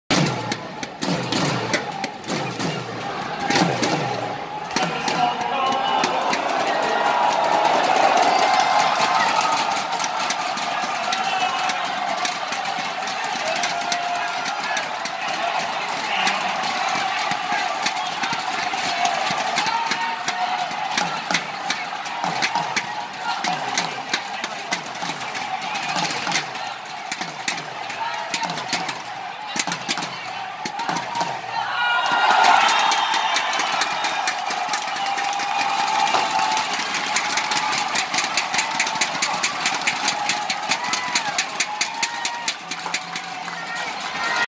Kevin Sanjaya Sukomuljo and Marcus Fernaldi Gideon vs Ong Yew Sin and Teo Ee Yi (winner reaction)
Kevin Sanjaya Sukomuljo and Marcus Fernaldi Gideon celebrate after defeating Malaysian  Ong Yew Sin and Teo Ee Yi 21-15, 21-16 at the Axiata Stadium, Kuala Lumpur Sport City